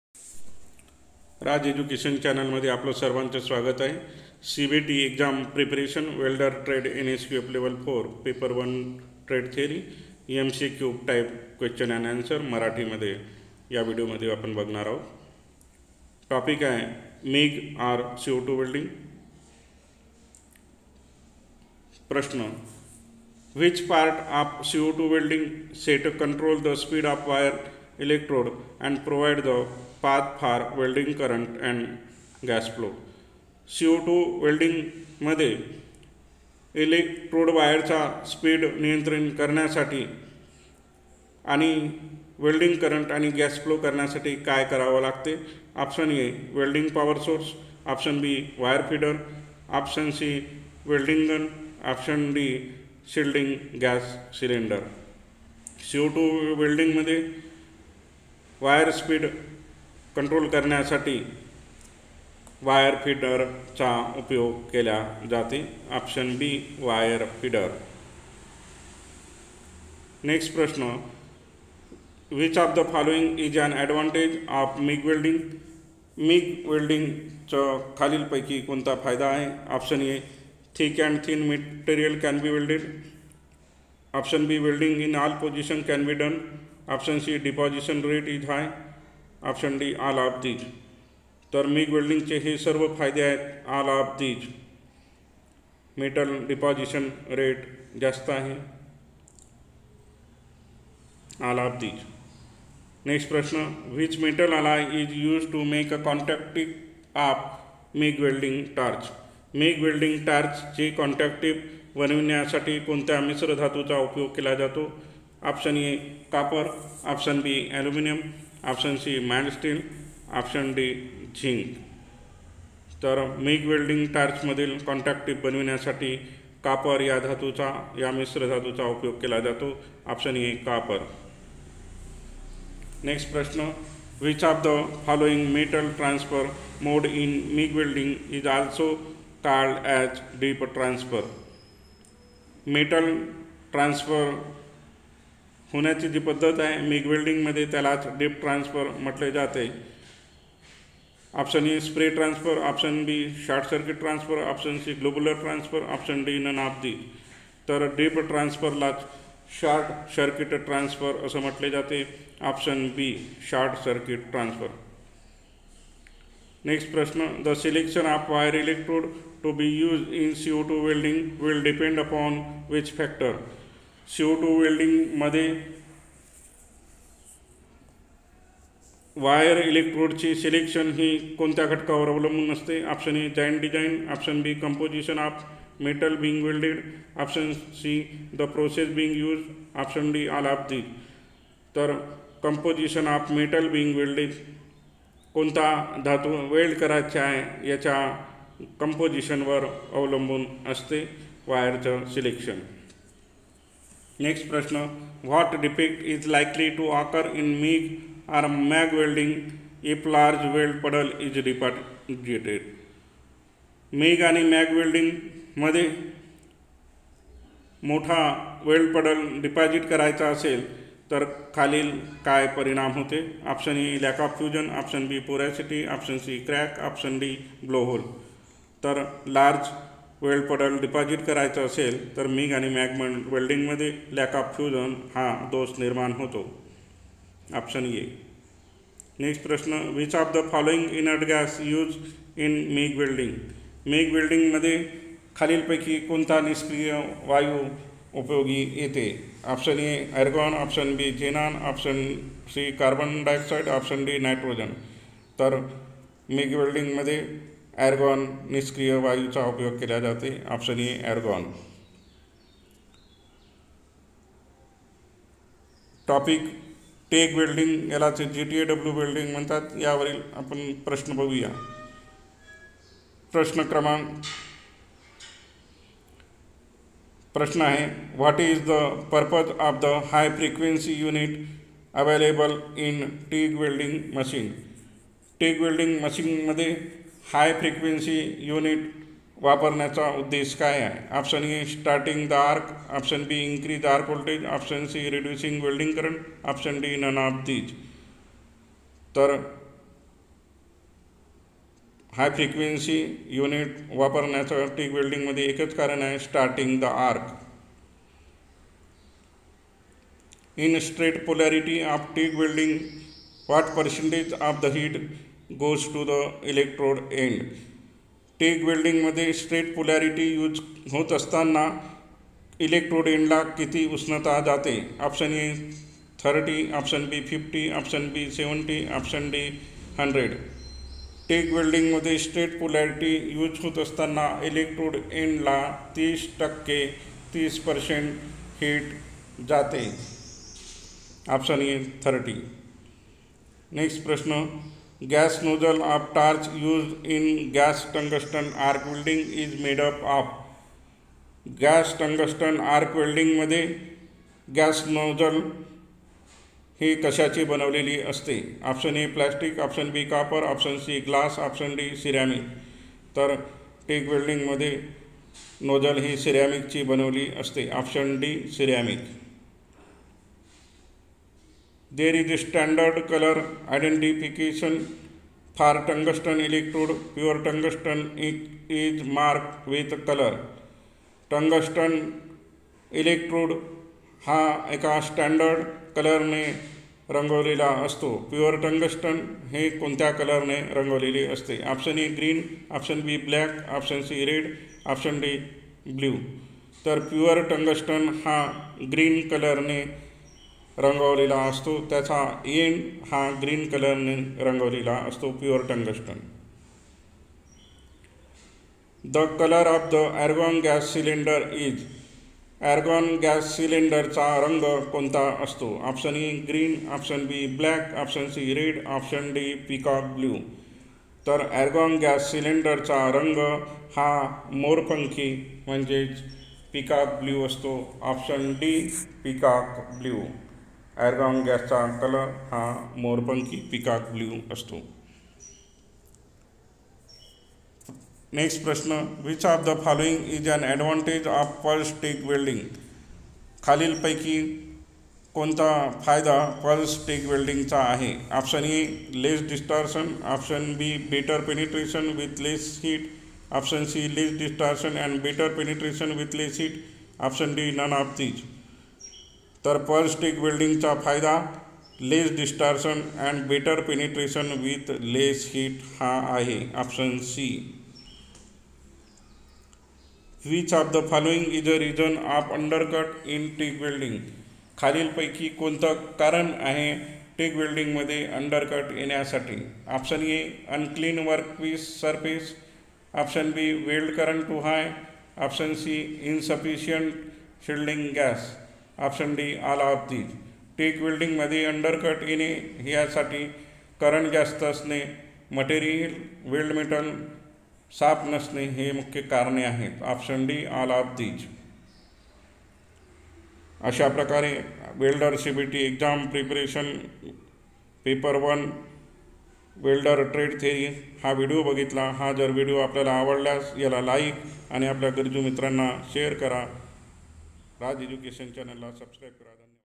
CBTExam,MonthlyTest-7,Paper-1,Trade Theory, Welder NSQFLevel-4,MCQ@Raj_Education_मराठी
CBT Exam Preparation Monthly Test-7,Paper-1,Trade Theory, Welder NSQF Level-4,
Topic :- MIG/GMAW or MAG/CO2 Welding
             TIG/GTAW Welding